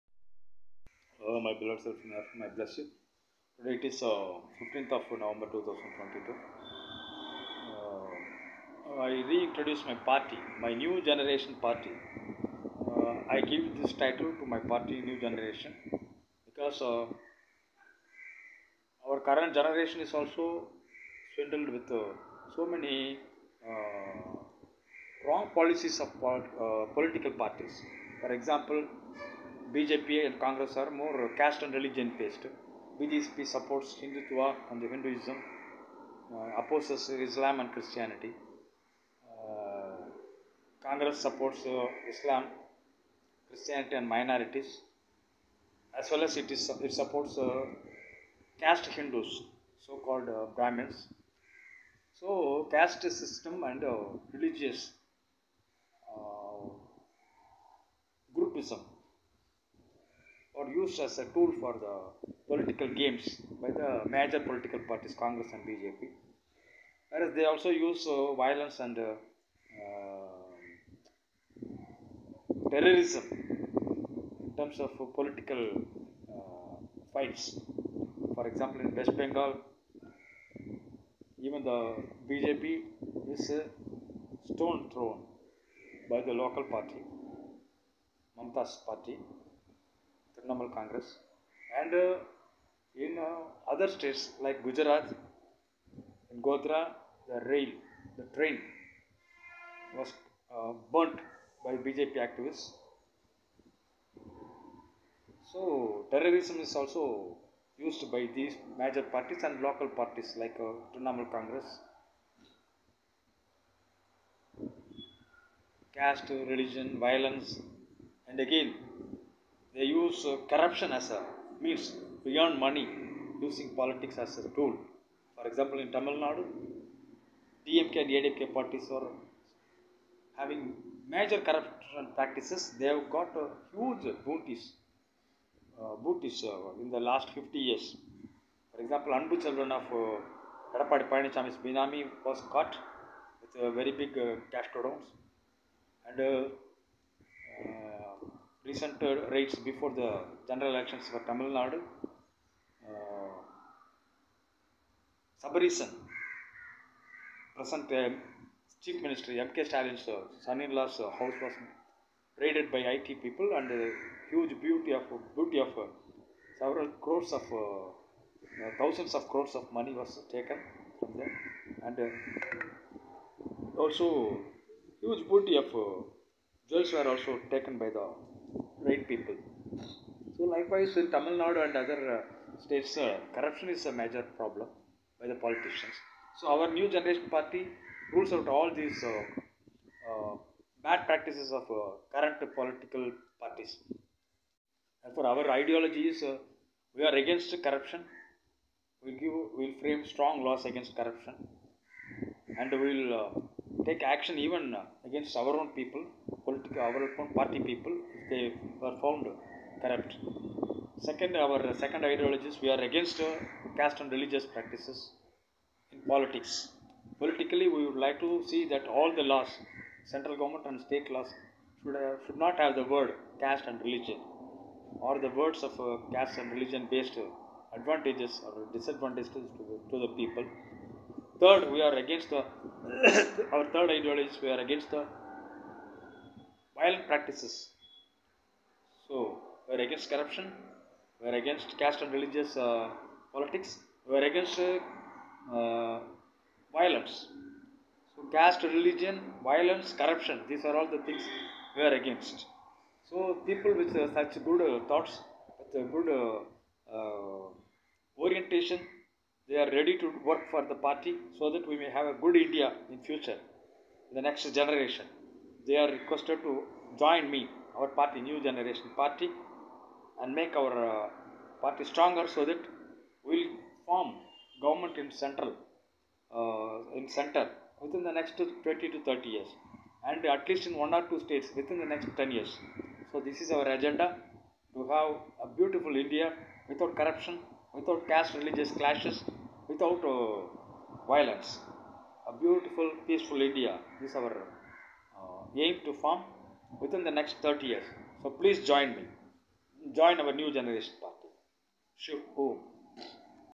An invitation to join 'New Generation Party'.
Divine Voice of Swamy Devarajan dt.15 11 2022.To join our New Generation Party' give a issrd call to me vide: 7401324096.